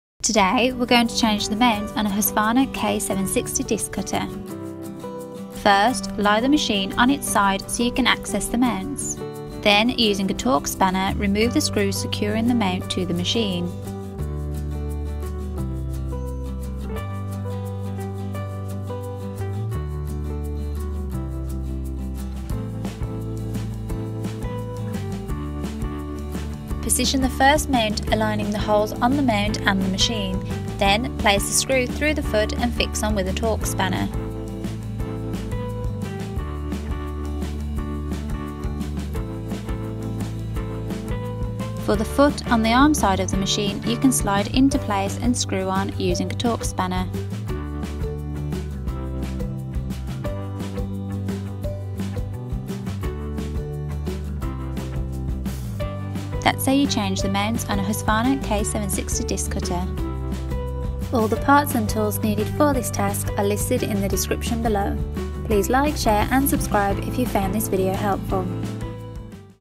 How to Change the Mounts on a Husqvarna K760 Disc Cutter | L&S Engineers
In this video, we show you how to change the Mounts on a Husqvarna K760. Click here for a full list of parts, tools & transcript!

Parts

Anti Vibration Mount for Husqvarna K760 Disc Cutters - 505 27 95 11

Tools
Combination Tool for Husqvarna K760

Health & Safety Warning Please ensure that you have safely prepared the machine and workspace. If you are unsure about any aspect of the task please seek professional advice.

Transcript

Today we're going to change the mounts on a Husqvarna K760 Disc Cutter. Mounts can easily be broken whilst using the machine, so it's handy to know how to change them. First lie the machine on it's side so you can access the mounts. Then using a torx spanner remove the screw securing the mount to the machine. The mount located on the arm side of the machine can slide off. The mount on the other side has to be pulled from the machine. Position the first mount aligning the holes on the mount and the machine, then place the screw through the foot and fix on with a torx spanner. For the foot on the arm side of the machine, you can slide into place and screw on using a torx spanner. That's how you change the mounts on a Husqvarna K760 Disc Cutter.

Please note L&S is a UK based trading company, please see our website for information on delivery to other countries.